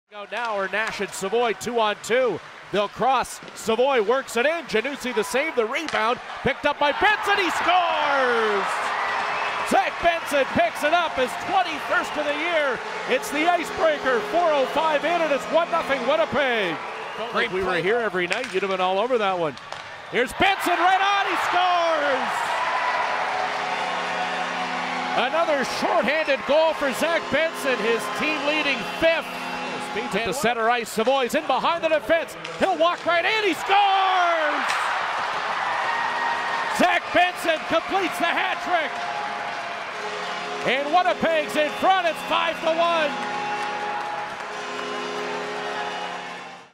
WHL Hat-Tricks - Zach Benson
Winnipeg ICE forward Zach Benson recorded a hat-trick January 8, 2023 versus the Portland Winterhawks.

Feeling social?